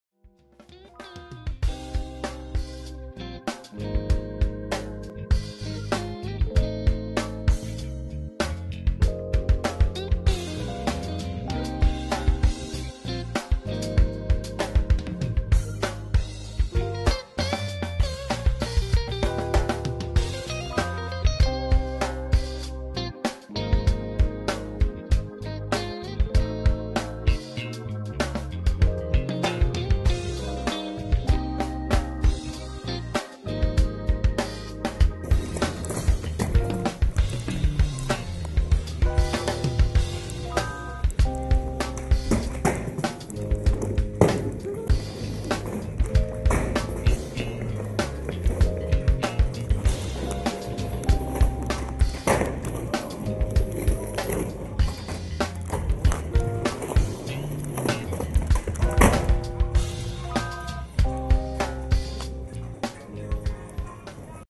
SPOTAKA SKTEBOARD PARK VIDEO REPORT 2016,05,13 DJi OSMO
DJI OSMOを使用してスポパーで撮影してきました！
KOTORA MITANIの鋭いスケートをどうぞ！